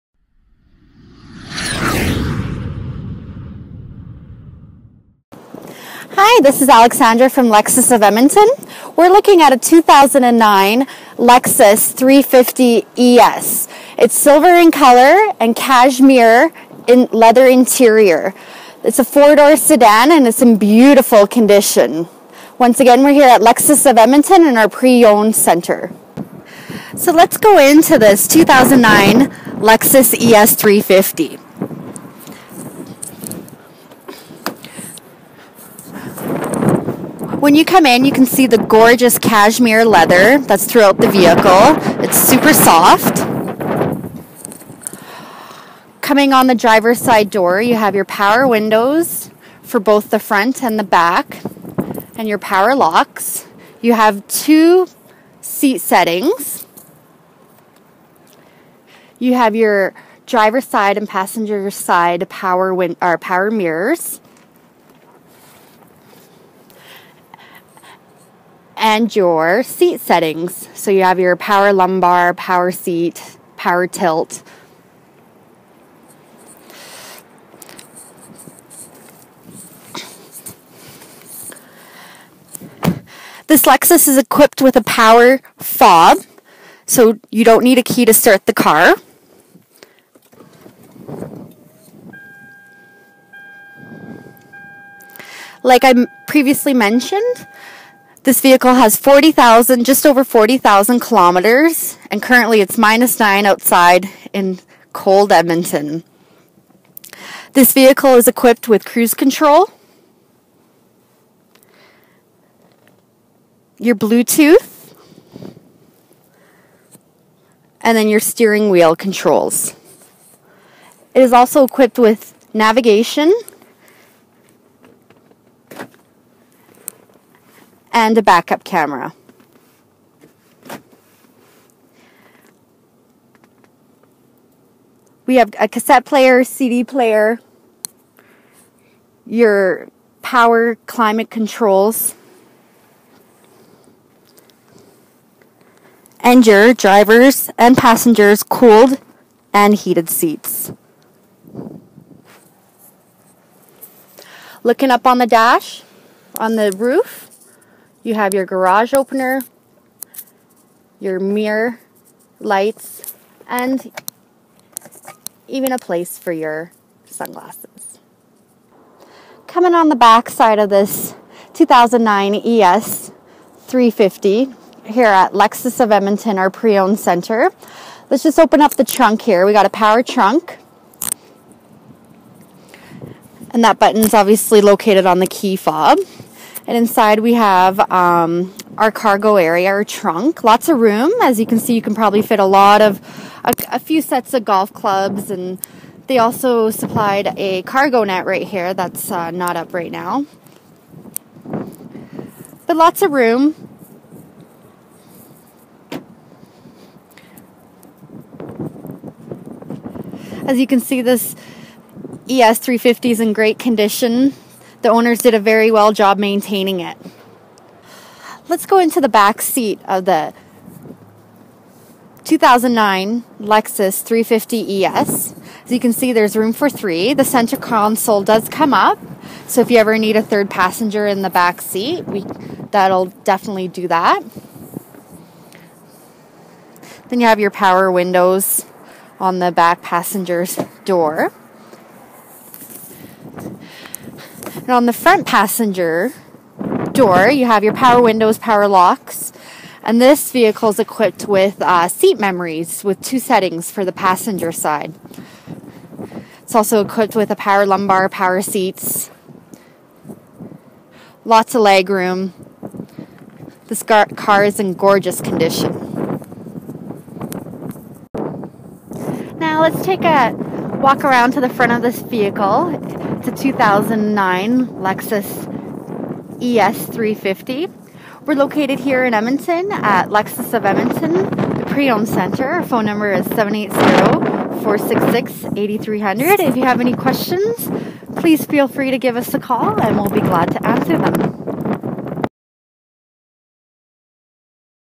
2009 Lexus ES 350 Sedan| Lexus of Edmonton
This is a 2009 Lexus ES 350 4dr Sdn 4 Door Car Front Wheel Drive with 6-Speed A/T transmission Silver[Moon Shell Mica] color and Cashmere interior color.

This video is recorded and uploaded by cDemo Mobile Inspector.                             Detailed vehicle info with more photo(s) is available